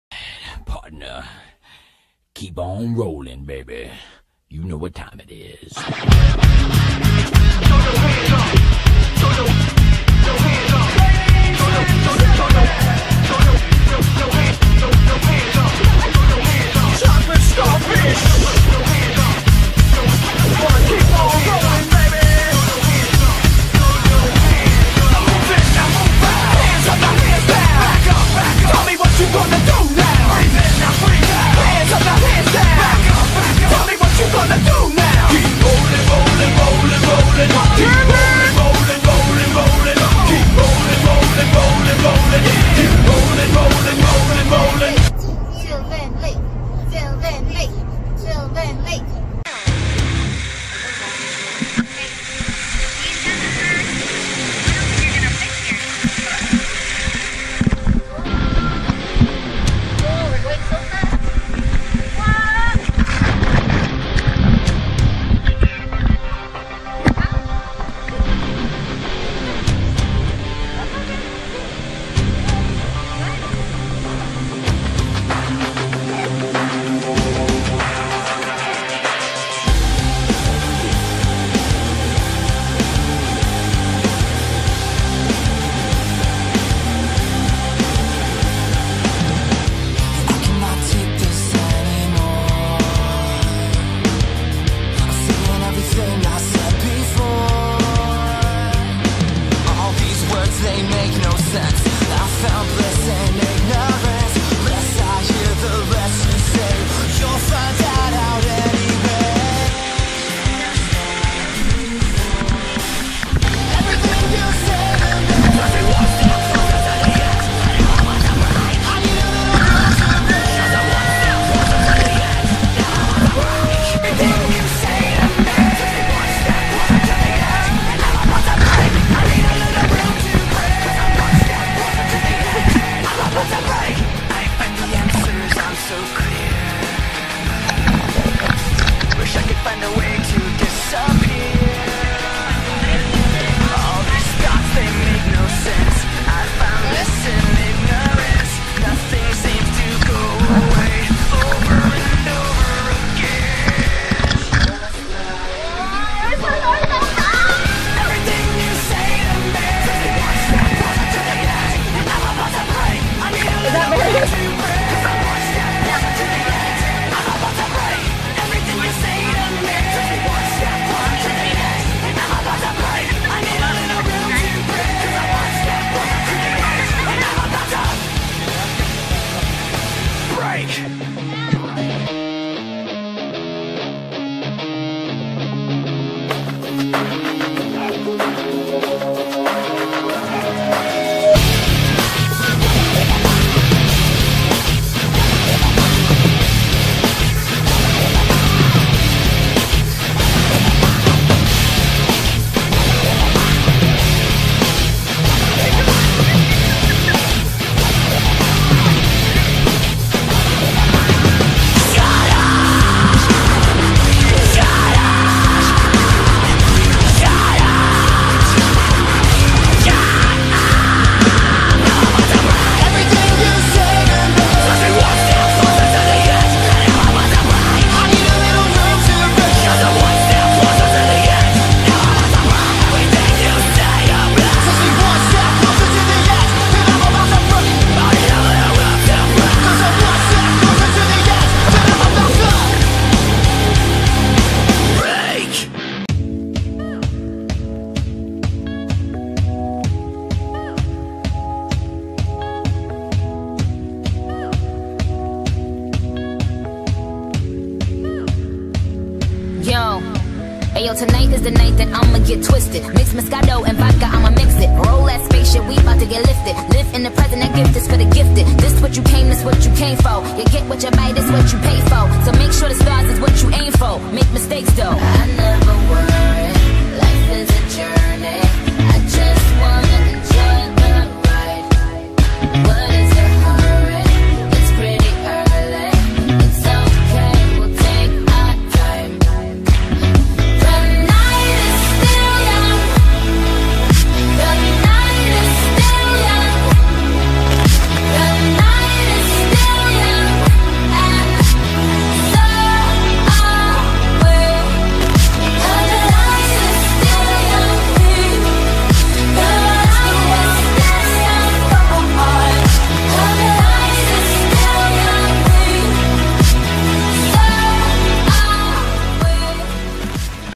Go Karts at Sylvan Lake to Hotel and Waterslides at Red Deer | WE THE MUTTONS vid#285
July 10, 2020 - Friday - And this was where we found out that Kaydence LOVES to go down fast on big waterslides !!!